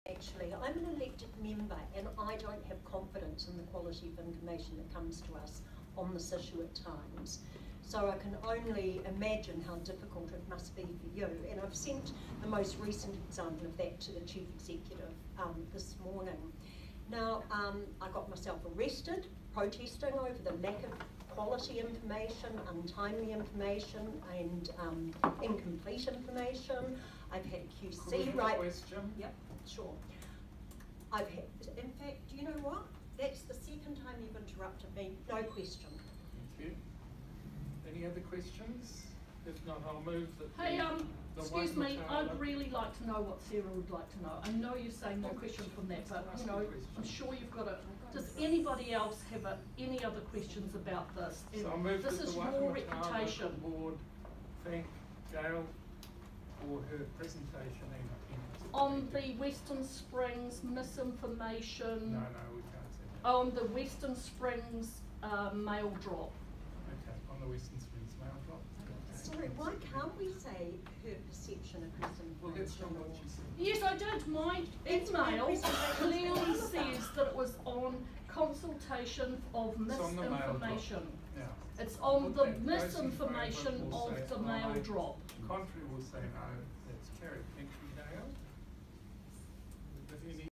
Member Sarah Trotman tries to ask Gael Baldock a Question 18.05.21
Waitemata Local Board Meeting 18.05.21
(1.31 minutes)